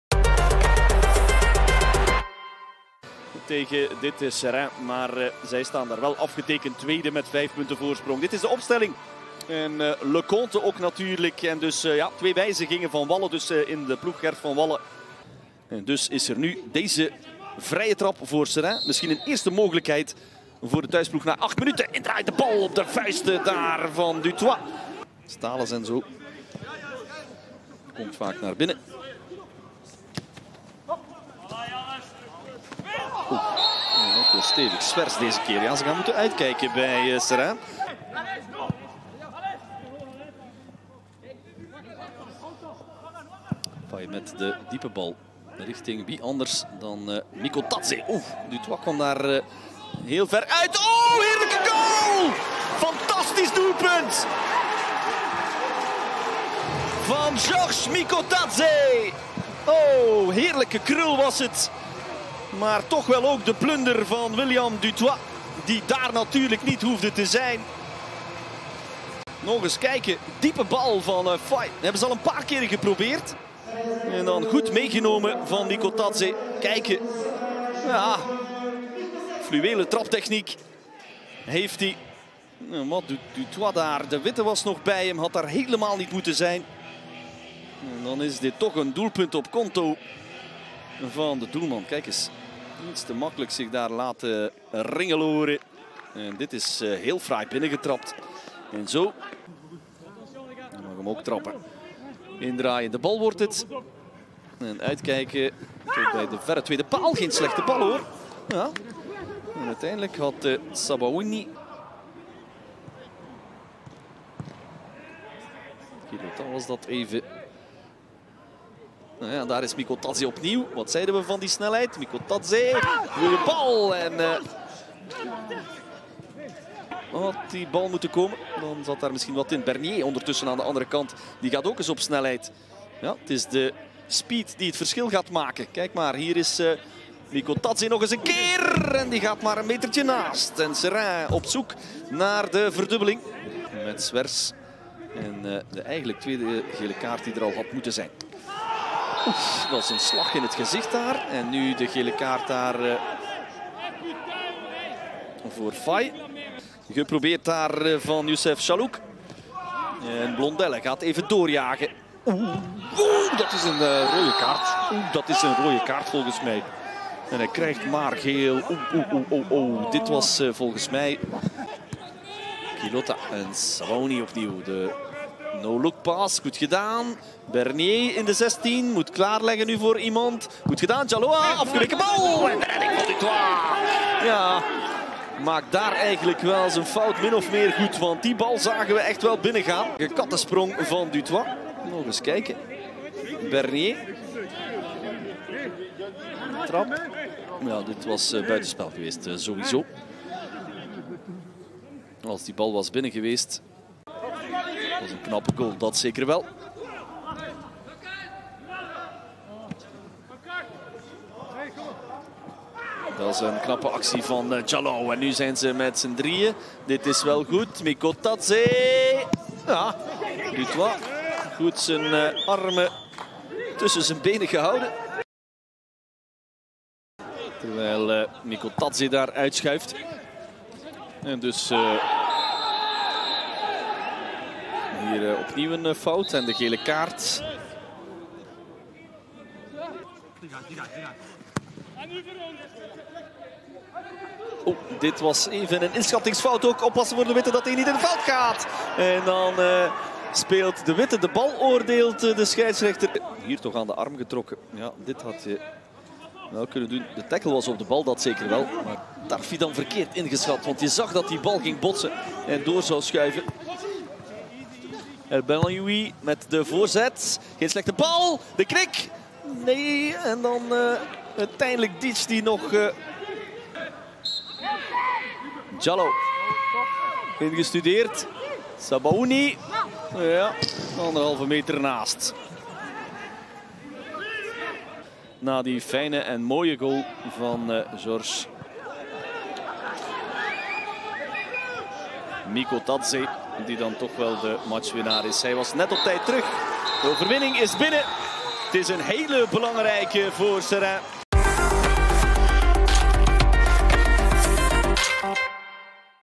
RFC Seraing - KMSK Deinze hoogtepunten
Hoogtepunten van de wedstrijd tussen RFC Seraing en KMSK Deinze op speeldag 26 van de 1B Pro League. (10/04/2021)